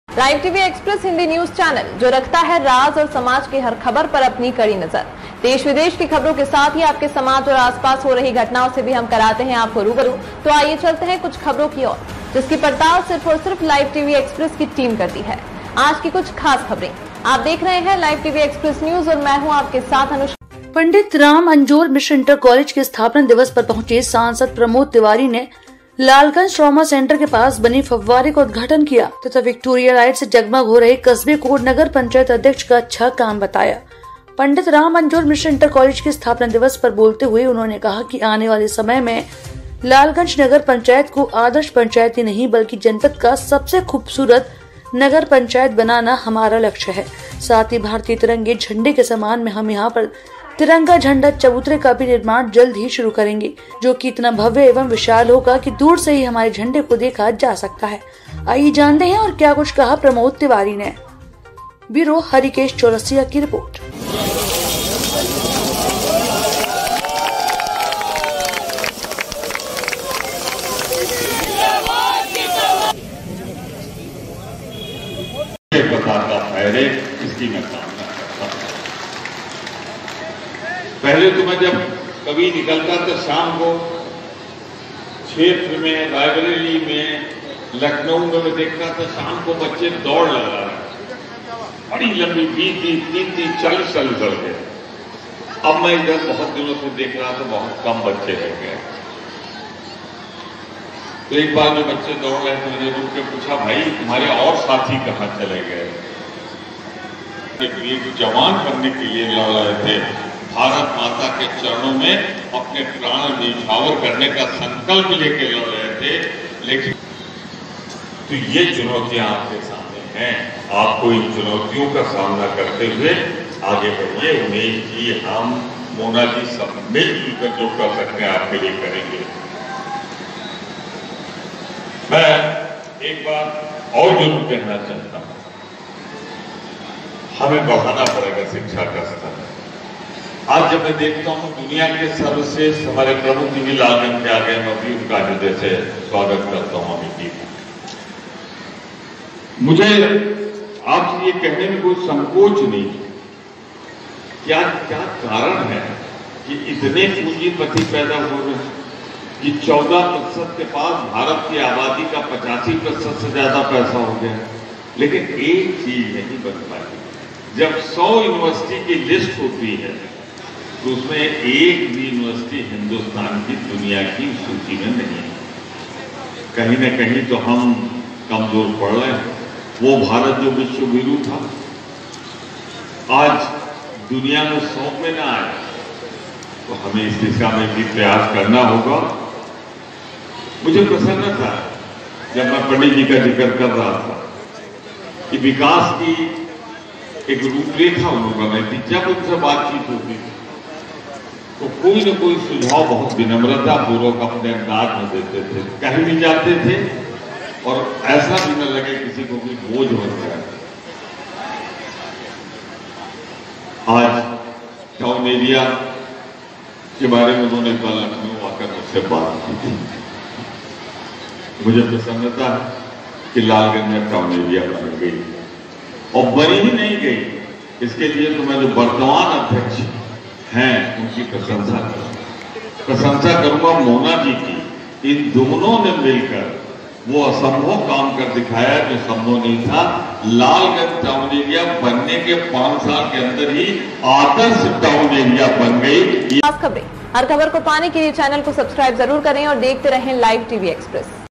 लालगंज ट्रामा सेंटर के पास बने फव्वारे का उद्घाटन किया | LIVE TV EXPRESS
डित राम अंजोर मिश्र इंटर कॉलेज के स्थापना दिवस पर पहुंचे सांसद प्रमोद तिवारी ने लालगंज ट्रामा सेंटर के पास बने फव्वारे का उद्घाटन किया तथा विक्टोरिया लाइट से जगमग हो रहे कस्बे को नगर पंचायत अध्यक्ष का अच्छा काम बताया।